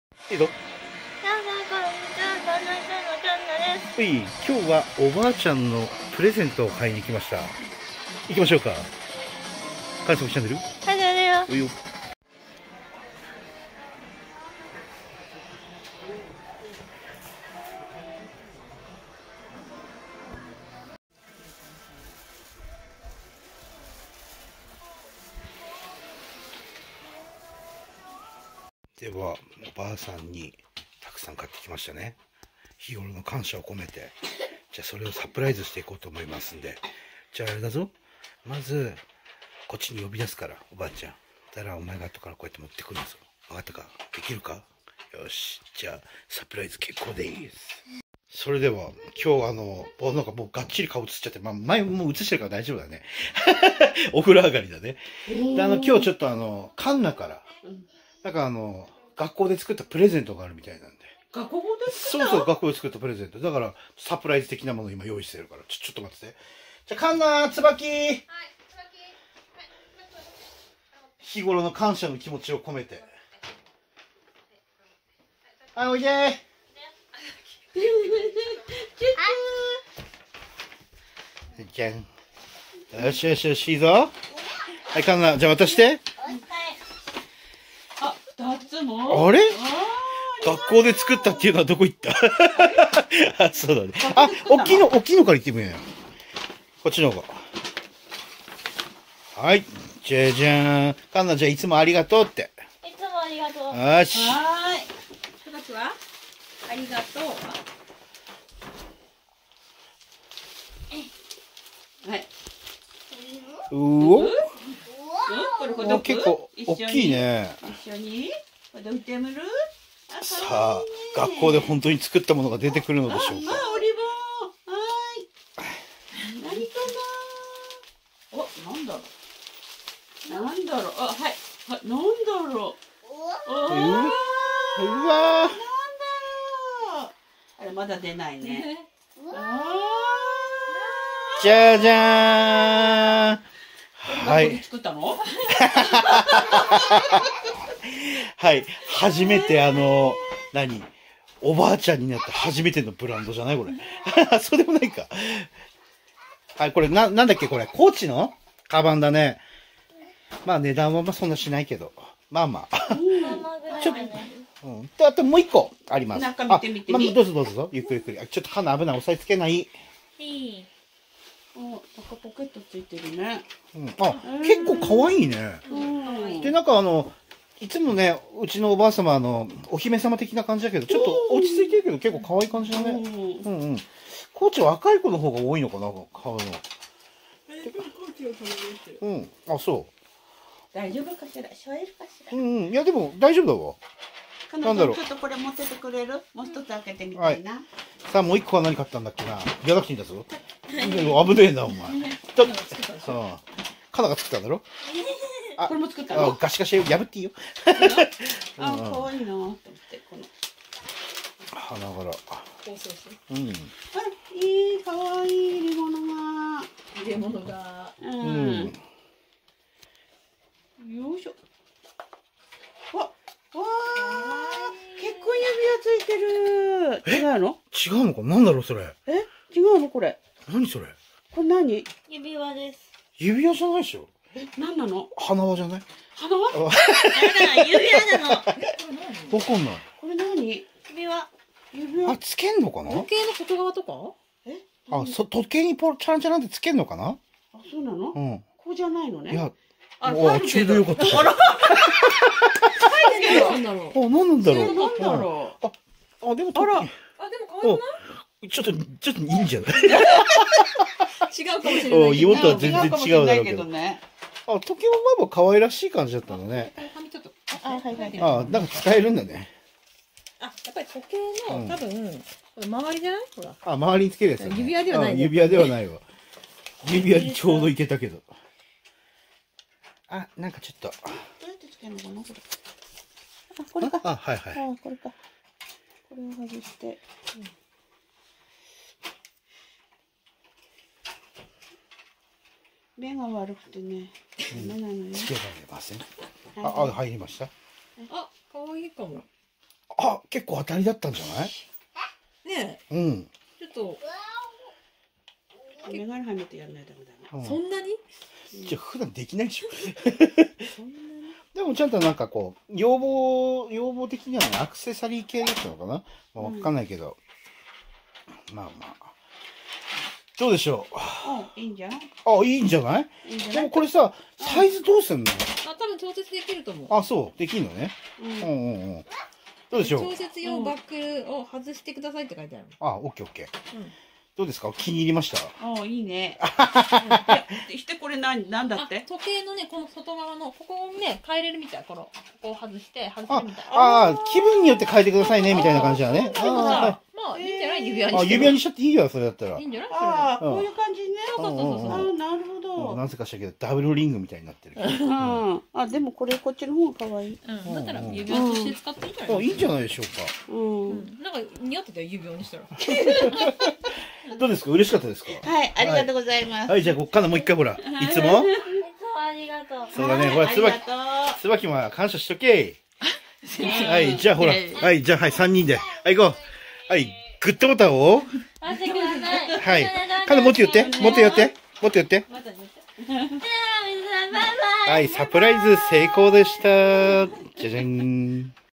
おばあちゃんに高級バッグを買ってみた COACH（コーチ）のバックを買ったらどんな反応するのでしょう 時計高級ブランド品 ドン・キホーテ鞄カバン サプライズどっきりドッキリ お婆ちゃんにイタズラ
喜んでくれたのかな？